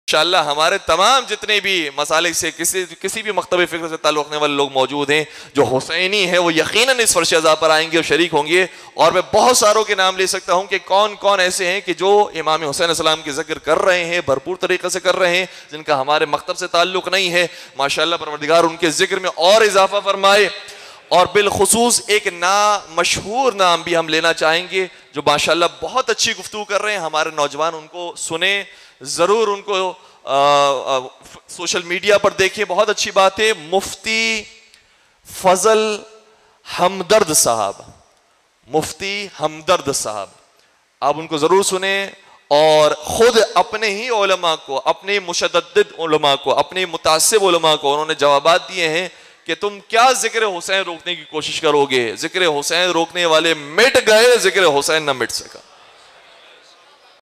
Allama Taqi Mehdavi Praises Mufti Fazal Hamdard In Majlis
شیعہ عالم دین نے اپنے مجمع میں سنی عالم دین کی کهل کر تعریف کردی.
خدا علم و محبت کے سفر کو جاری رکھے.